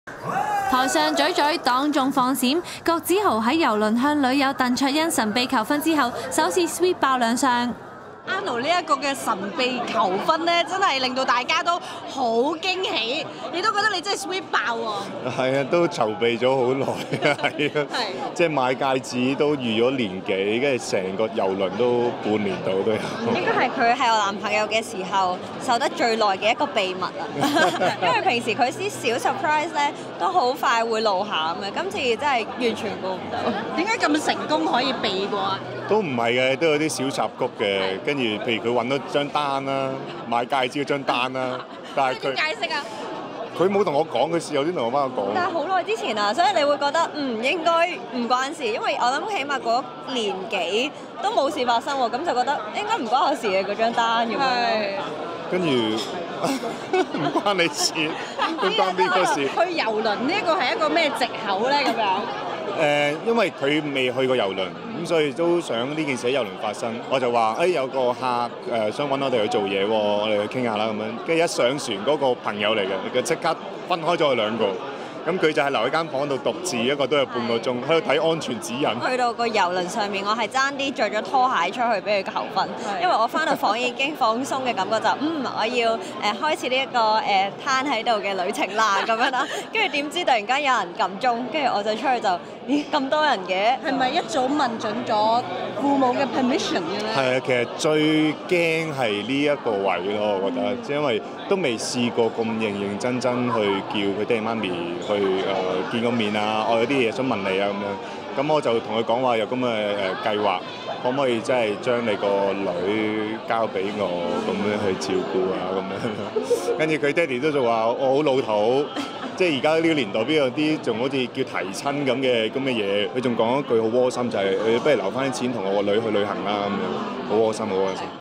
郭子豪鄧卓殷夫妻檔出席活動 台上親吻幸福感滿滿
郭子豪、鄧卓殷公開婚訊後，首次以夫妻檔出席活動，二人於台上親吻對方，浪漫滿滿，羨煞旁人！

二人回應大家的祝福，亦大方分享遊輪求婚時的點滴。訪問中二人的幸福感滿瀉，「隔住個mon都覺得甜」！